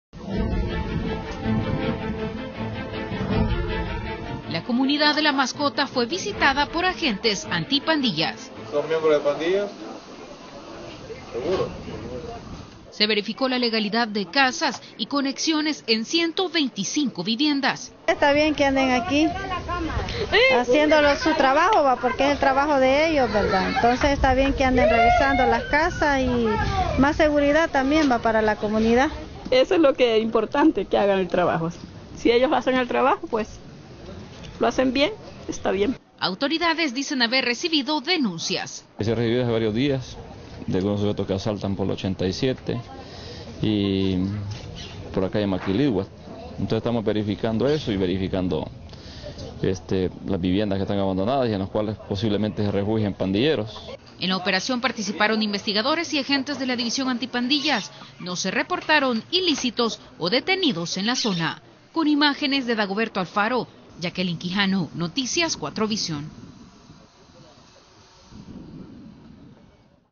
Operativo "Casa Segura".
La PNC realizo un operativo "Casa Segura" en la comunidad La Mascota de S.S. (16/01/2014).